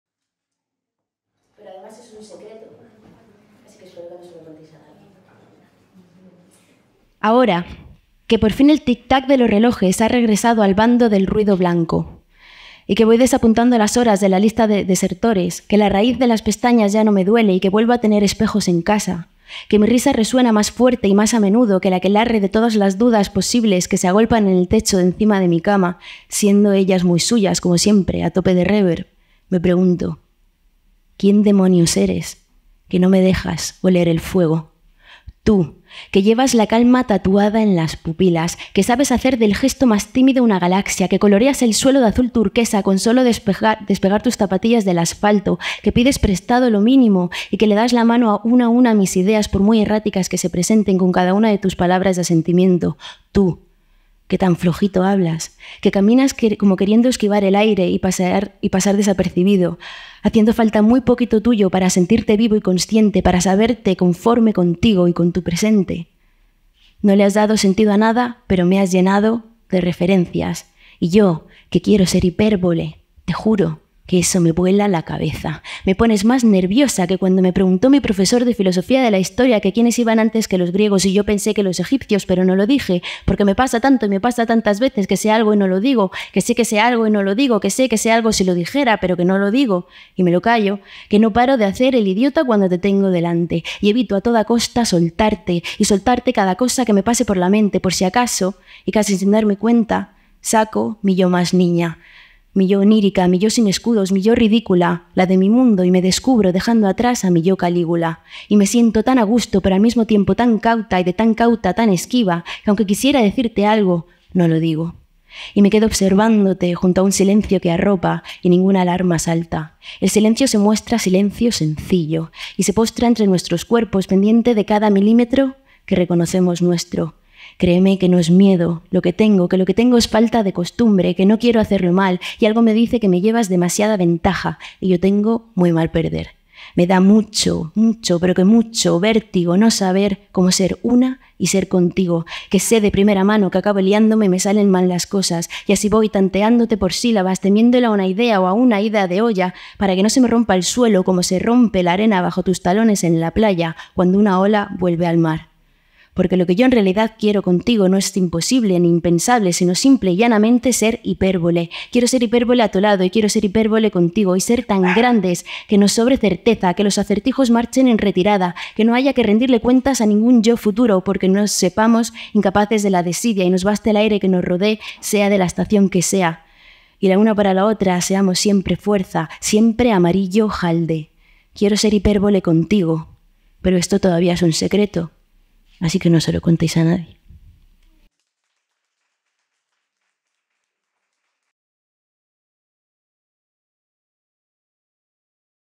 CAROLINA TREBOADA - Amarillo jalde
AMARILLO JALDE, es un poema de Carolina Treboada que recitó en Aleatorio Bar durante el III encuentro de Escaparate de Poesía en el ciclo del mismo nombre que, desde este colectivo, estamos llevando a cabo durante 2023.

En ese marco es en el que estamos grabando esta serie de poemas.

Carolina Treboada reside en Sevilla, aunque ha vivido en muchos sitos y pasa a menudo por Madrid. Ha participado en varios Poetry Slam, y además de recitar, rapea. Hay contenidos de los dos tipos en su canal de Spotify.